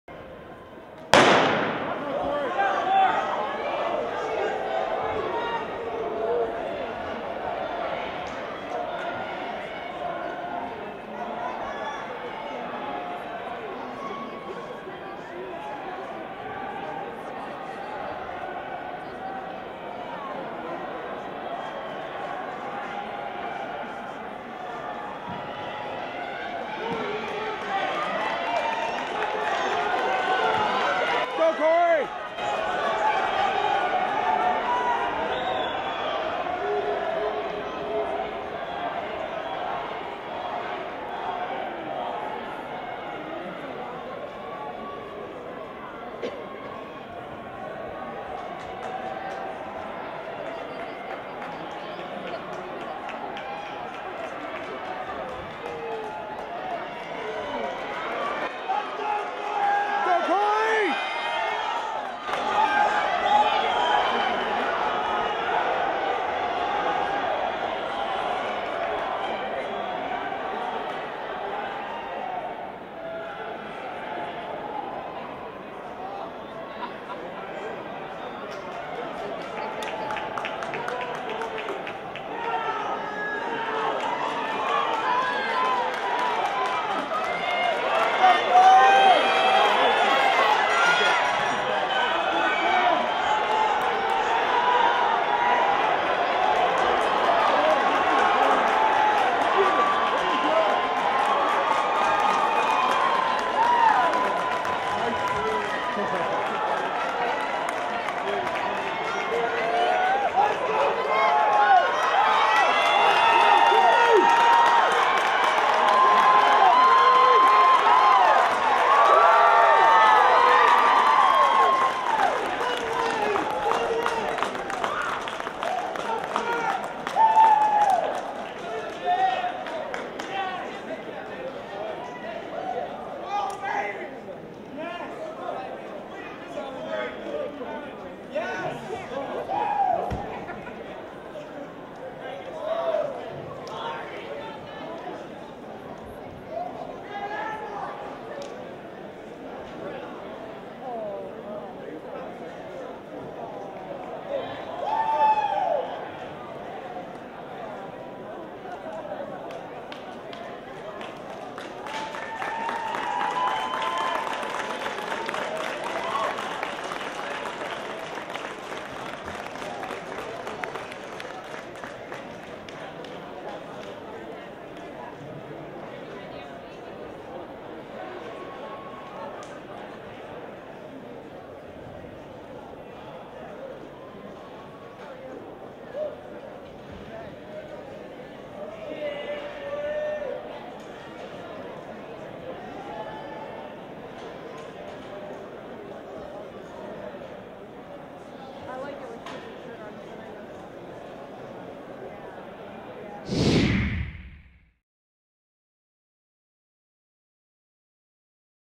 2017 CCHS State Indoor Track Meet - 800m Cory Myers
1st place finish for Cory Myers in the 800m during the Wyoming State Indoor track meet in Gillette, Wyoming in 2017.

In This Moment Productions
@ITMP_Sport - Twitter